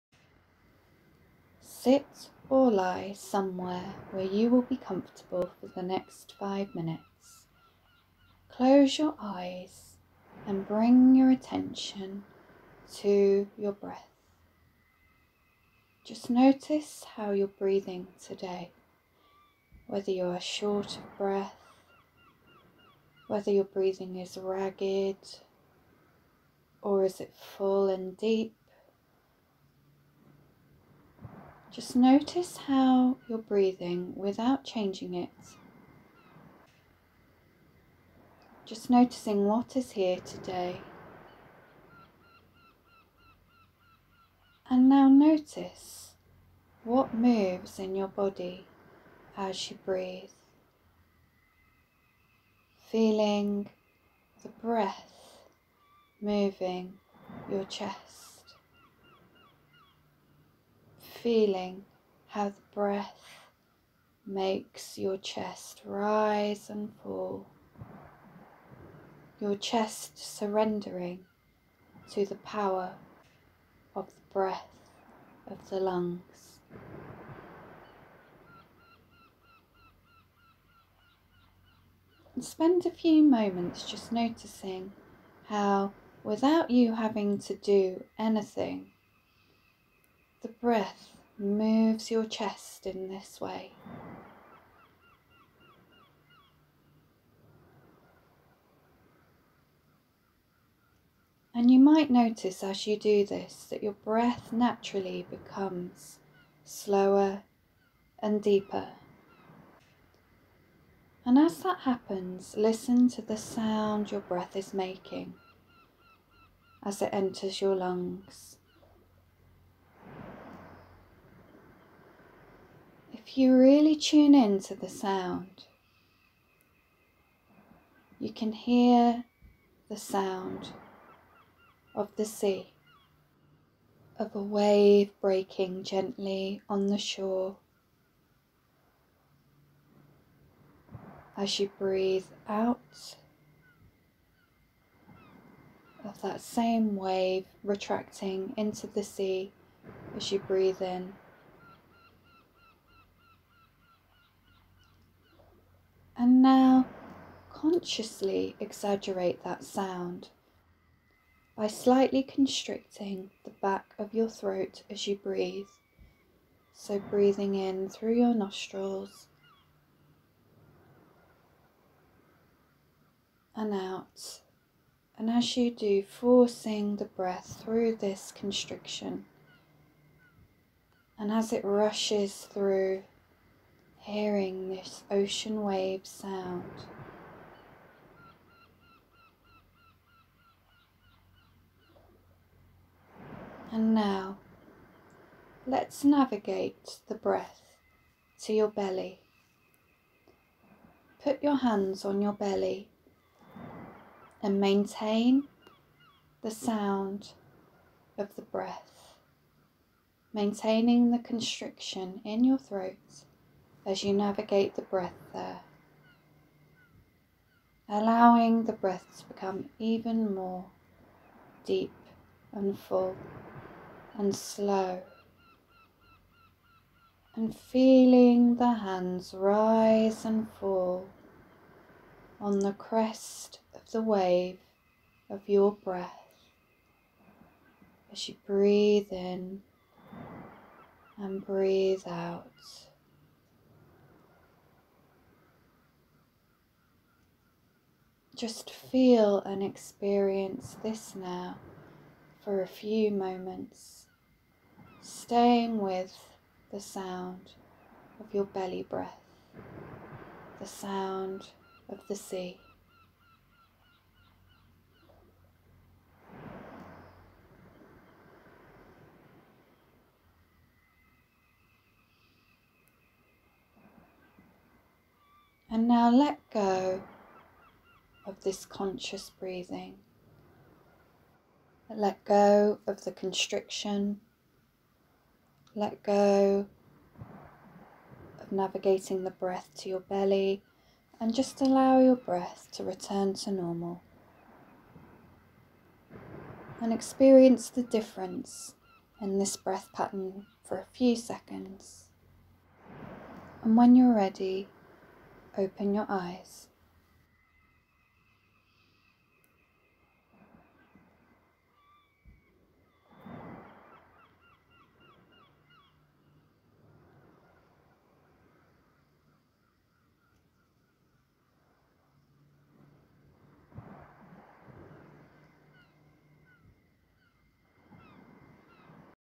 5 Minute Meditation: Ocean Breath
This intensely calming practise connects you to stillness as you follow the wave of your breath, accompanied by the soothing sounds of the ocean.

This practise is in honour of a great yoga teacher Jackie Jones whose voice, technique and calm instruction I hear echoing in mine. This one's for you, Jackie.